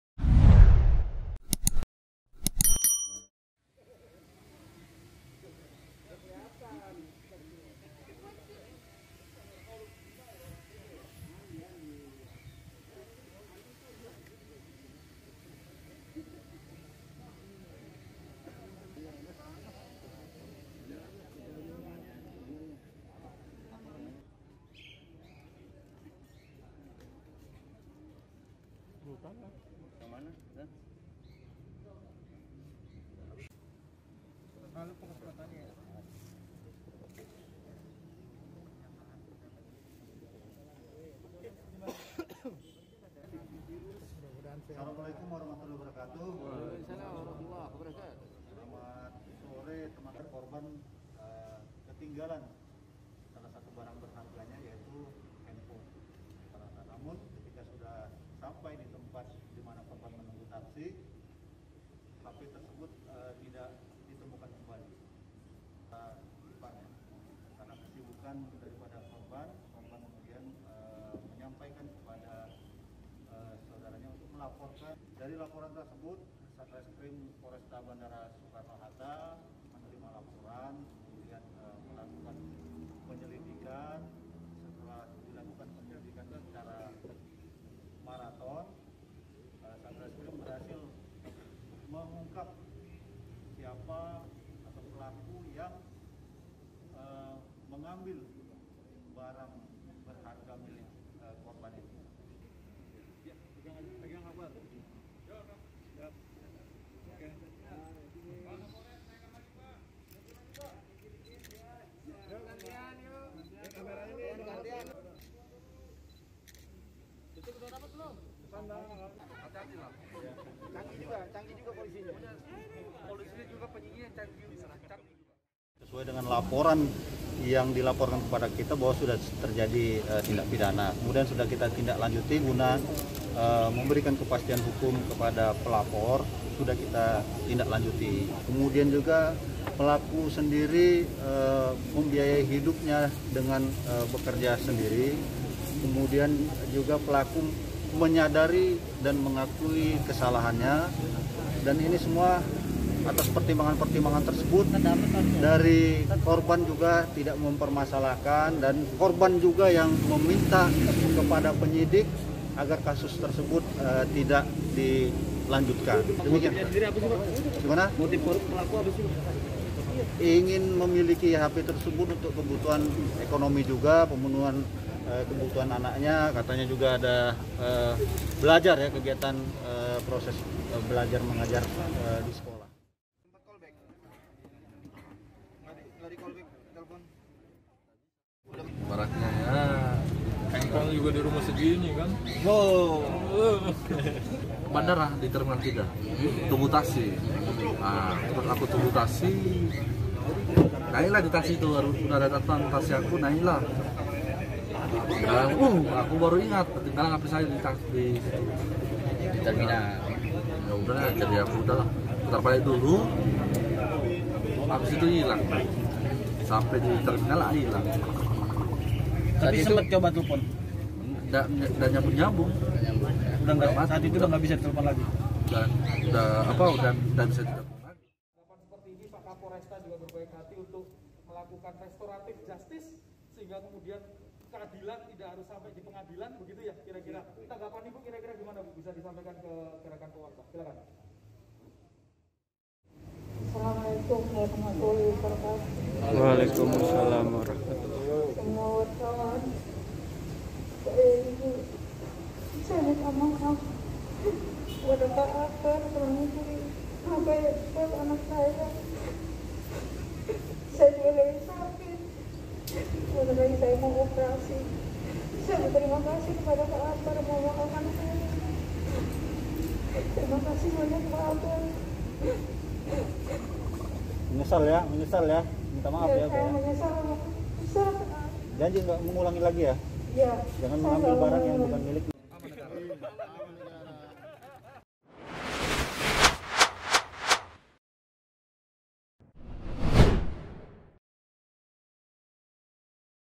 TANGERANG, PELAKU PENCURI HP SELEBGRAM AJUDAN PRIBADI DILEPAS KARENA KEMANUSIAAN
TANGERANG (Pos Kota) - Terapkan program Restorative Justice, Polresta Bandara Soekarno-Hatta melepas S (47), pelaku pencurian ponsel milik selebram Akbar PB atau biasa dikenal sebagai Ajudan Pribadi di Bandara Soetta, Kota Tangerang, Banten.

Kapolresta Bandara Soetta, Kombes Adi Ferdian mengatakan kejadian tersebut berawal saat korban baru tiba dari Makassar ke Jakarta pada Rabu (17/2/2021).

Saat itu korban menunggu taksi di area Gate 5 Kedatangan Bandara Soekarno-Hatta dan melepas jaket, telepon seluler.

"Saat taksi mengangkut, tanpa sadar korban ketinggalan barang berharganya, yaitu ponselnya," ujar Adi, Rabu (24/2/2021).